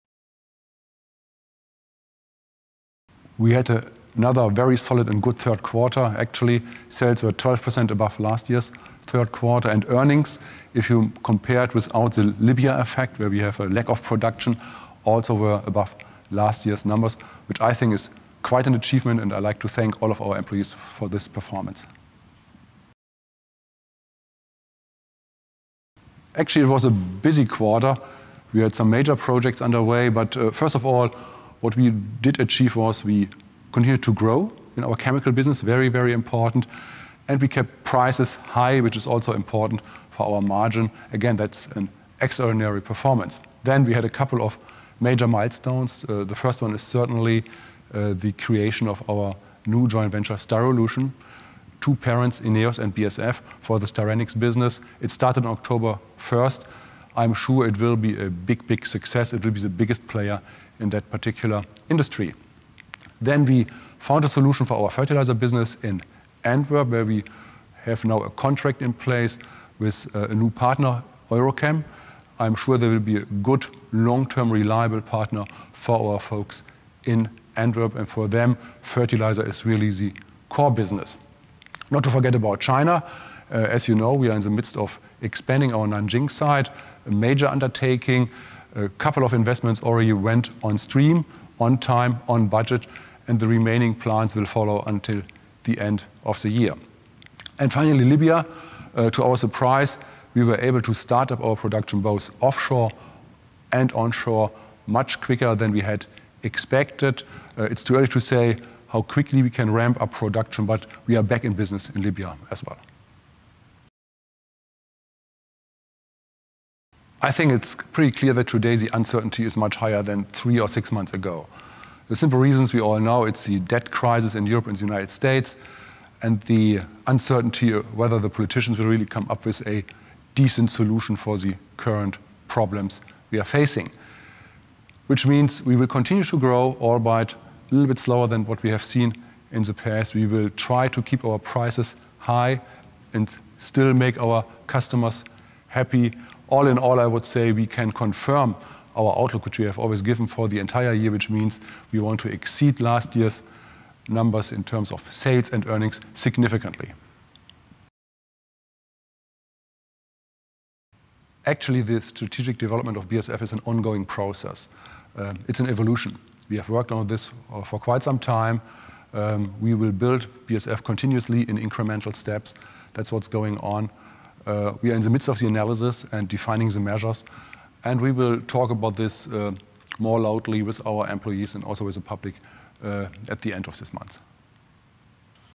BASF SE 2011 - Third-Quarter Report - Statements by CEO Dr. Kurt Bock on earnings
Free media footage and statements from BASF SE are available

Statements by CEO Dr. Kurt Bock
BASF with good earnings in 3rd quarter

3rd quarter 2011 of BASF
- Sales up 11.6% and EBIT before special items adjusted for contribution from
Libya above level of third quarter of 2010 (up 6%)

- Demand at high level

- Oil production in Libya recommenced in October

- Outlook for 2011: Sales and earnings still expected to significantly exceed previous year's level